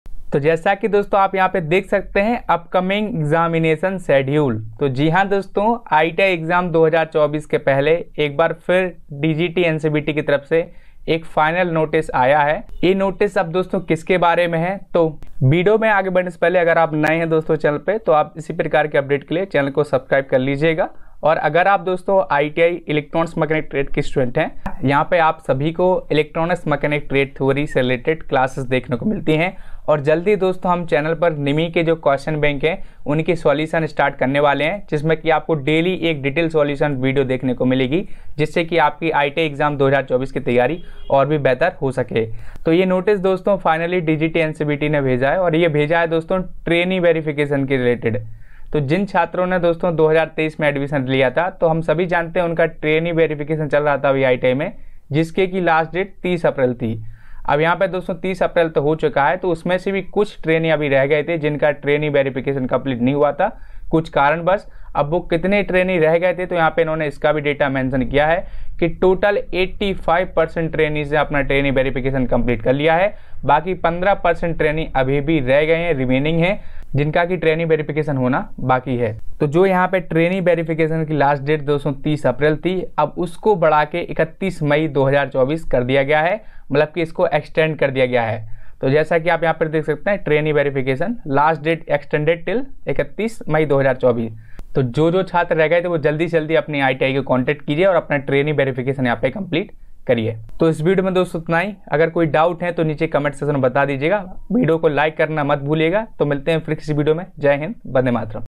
ITI Exam Update 2024 || DGT NCVT Final Notice for ITI Students
नमस्कार दोस्तों इस वीडियो में मैंने आपको ITI Exam 2024 DGT NCVT के Final Notice के बारे मे तो आप सभी इस video को पूरा देखें धन्यवाद दोस्तों जय हिंद